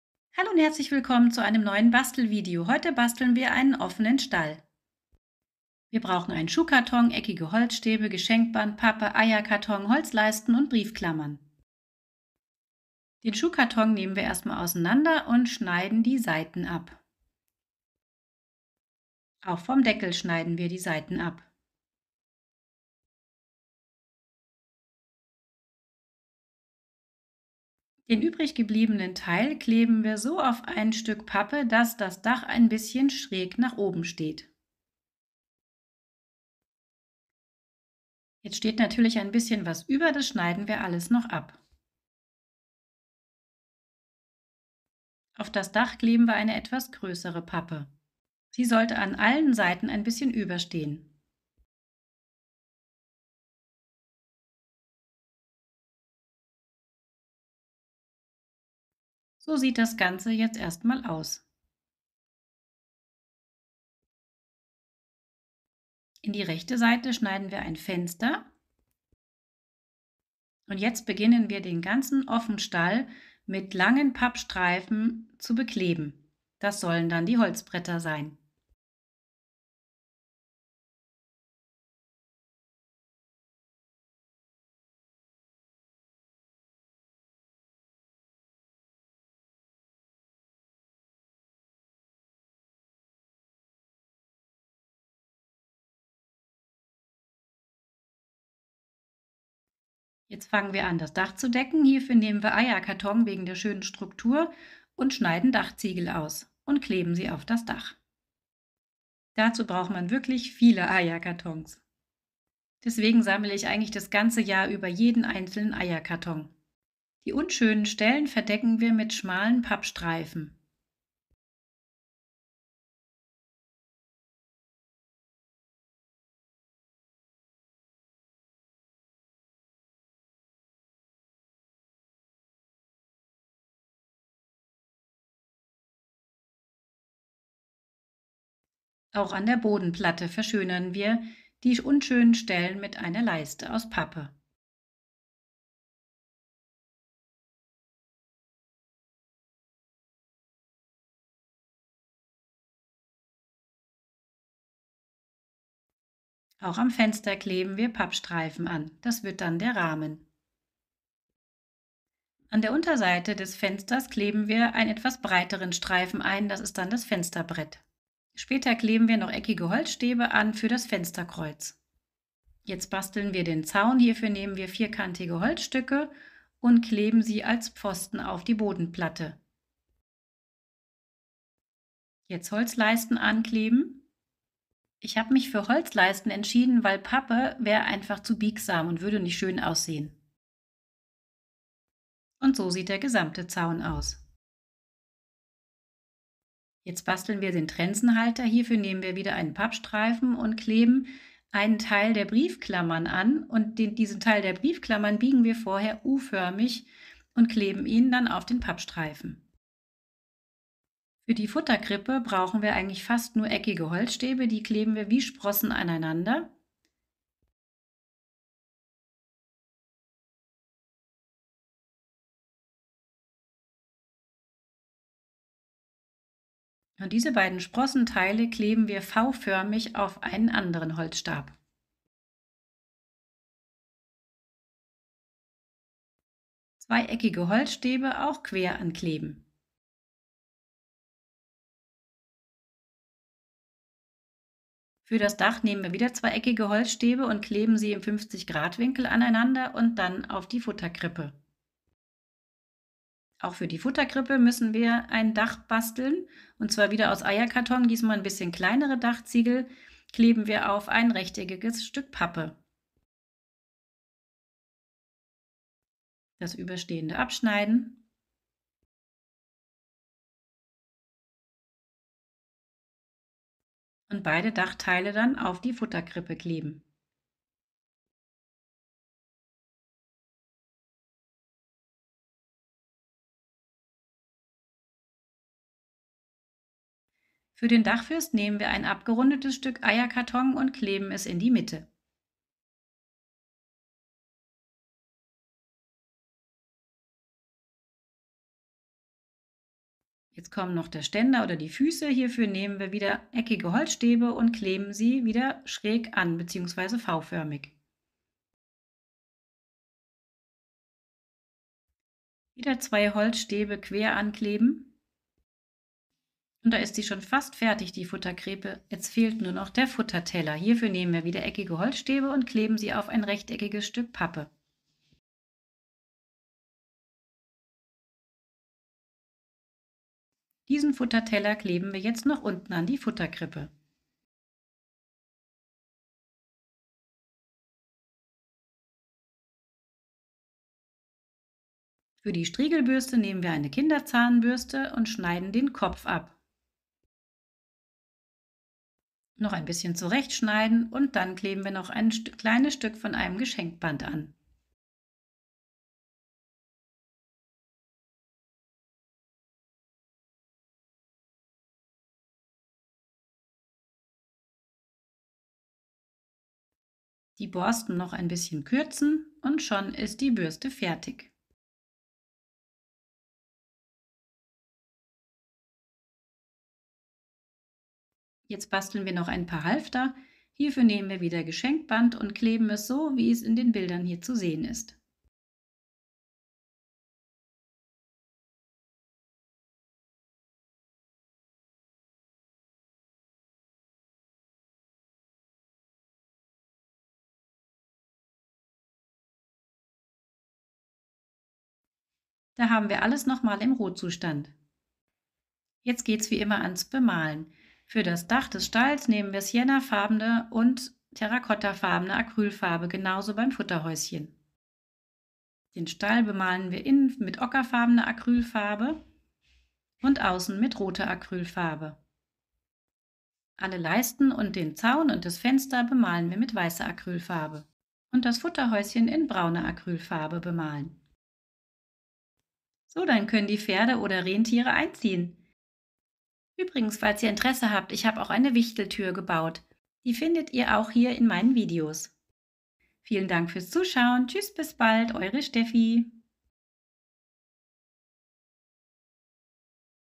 Tutorial: Mini-Rentierstall und eine Futterkrippe basteln / DIY: craft a miniature reindeer stable
Den Link zu meiner Bastelschere und meinem Heißkleber findet ihr in den Kommentaren.